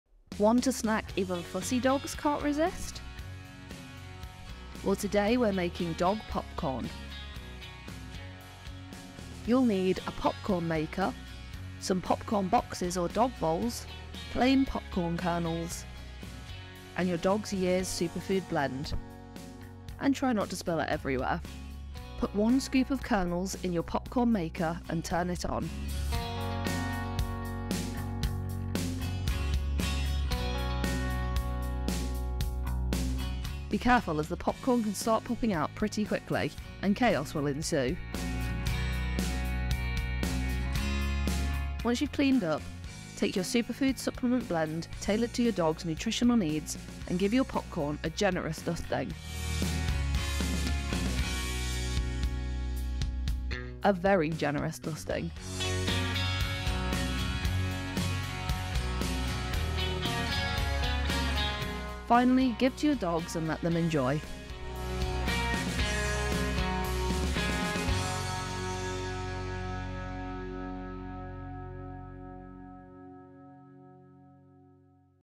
🍿🐶 Pupcorn Perfection: Discover the Secret Recipe to Doggy Popcorn Delight! 🍿🐶
In this video, we'll show you how to make a delicious and safe popcorn specifically designed for dogs. Our doggy popcorn is a healthy and flavorful snack that will have your furry friend wagging their tail with joy!

🍿 Doggy Popcorn Recipe: How to Make a Pawsitively Tasty and Safe Treat! 🍿

While traditional popcorn may not be suitable for dogs due to added seasonings and oils, our doggy popcorn recipe eliminates any potential hazards and focuses on healthy alternative toppings like our Years Superfood Supplement Blend. With this recipe, you can enjoy a bonding experience with your pup while ensuring their safety and satisfaction.

💙 Healthy Ingredients, Happy Dogs 💙

To find out more, visit our LINKTREE